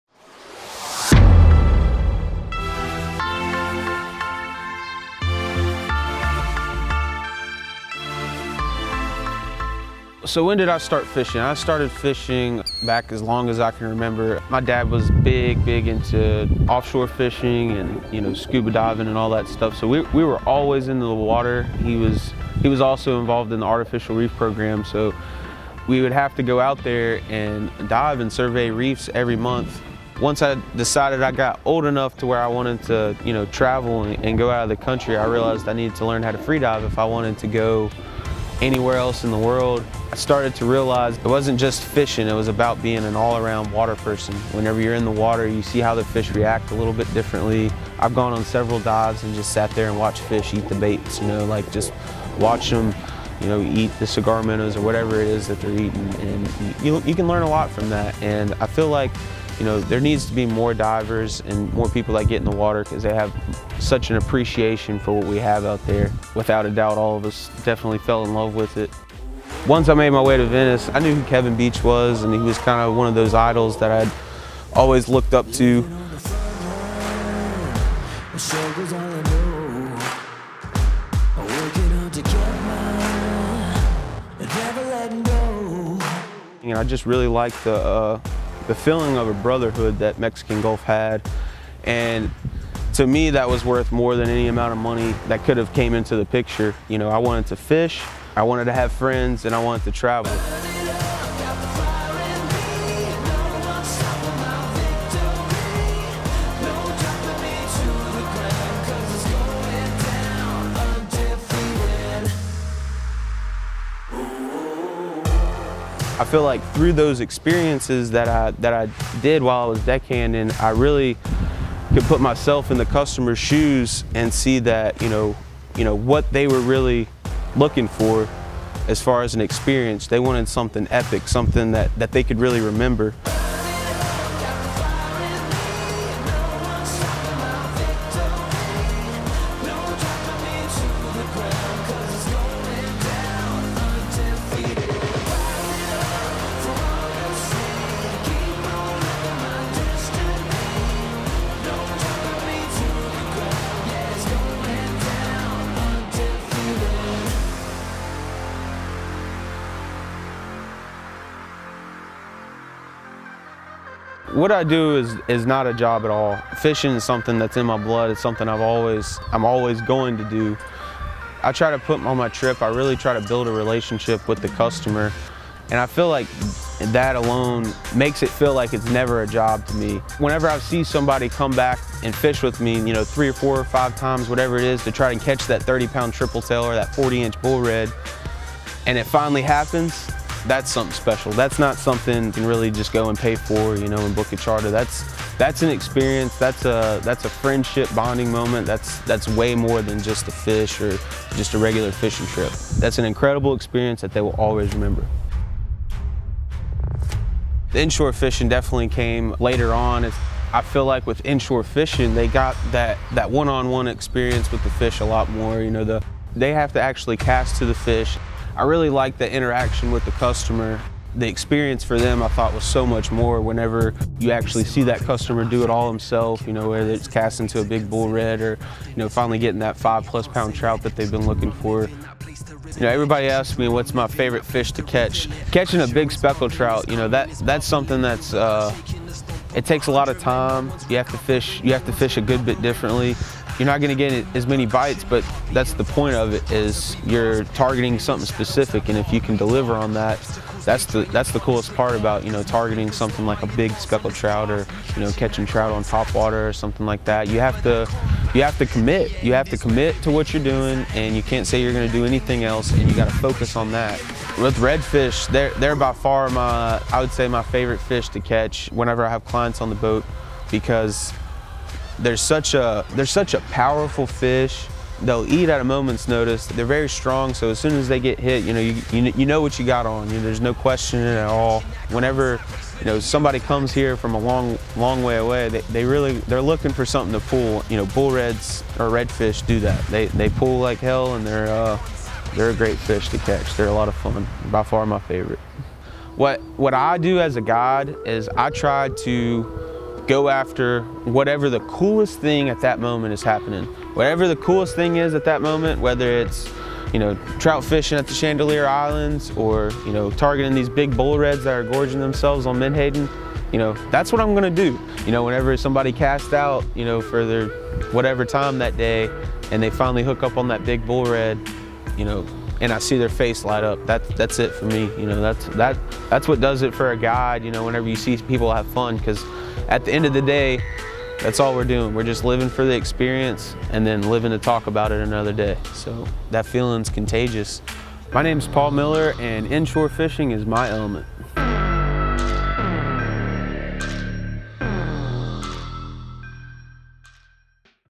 Paul Miller | Elements | Mexican Gulf Fishing Company, Venice Louisiana
From backwaters to bluewater, saltwater fishing is in Paul's blood. Ride along as we explore Paul's story of growing up along the coast,, his profession as a fishing guide, and why being on the water truly puts Paul in his Element.